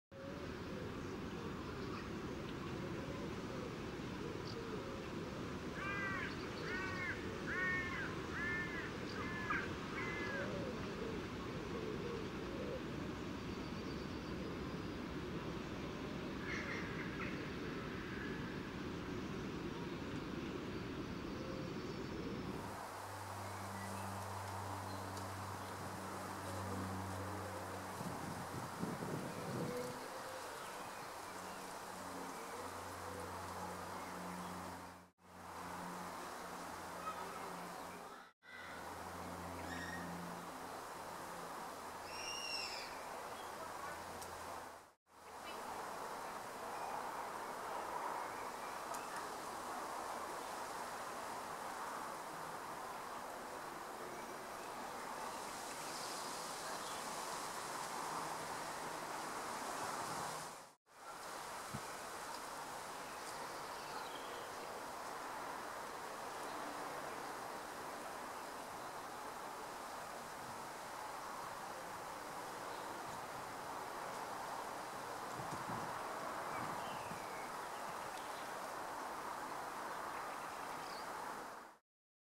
Solitary bees: Wool carder bee and Patchwork leafcutter bee
This clip begins with a Wool carder bee (Anthidium manicatum)  visiting purple toadflax in the orchard meadow and then shows a Patchwork leafcutter bee (Megachile centuncularis) nectaring on small scabious in the same meadow. This remarkable, small bee, is then shown lining a brood cell in a brick wall. For more of their story please see our blog.